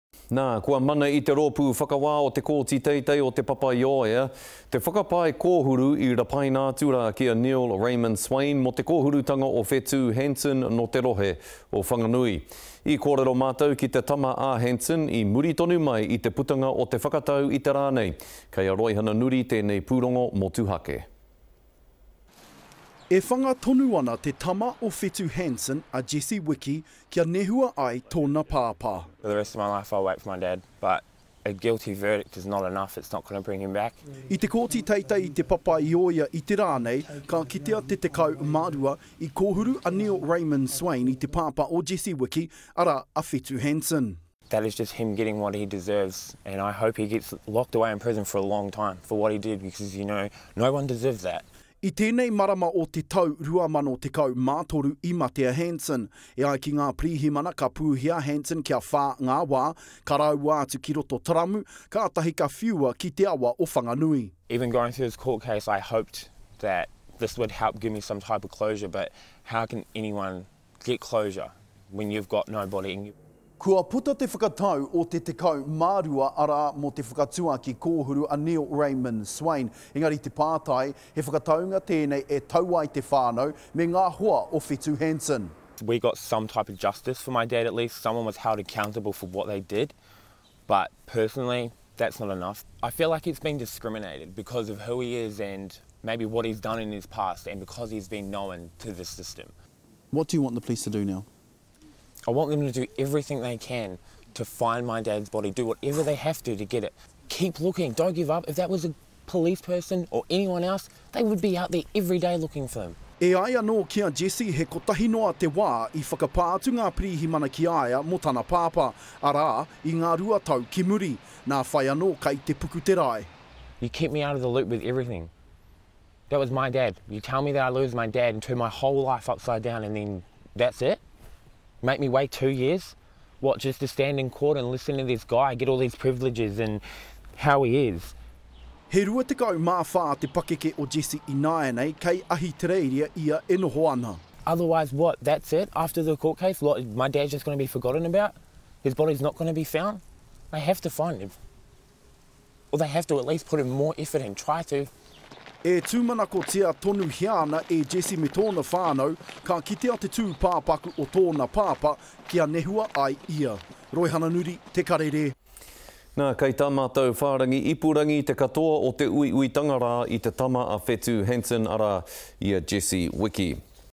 “Guilty verdict is not enough,” says murder victim’s son
Neil Raymond Swain was found guilty of murdering Whetu Hansen in the Palmerston North High Court today. Mr Hansen’s 24-year-old son Jesse Wiki spoke exclusively to our reporter Roihana Nuri following the more than three week trial.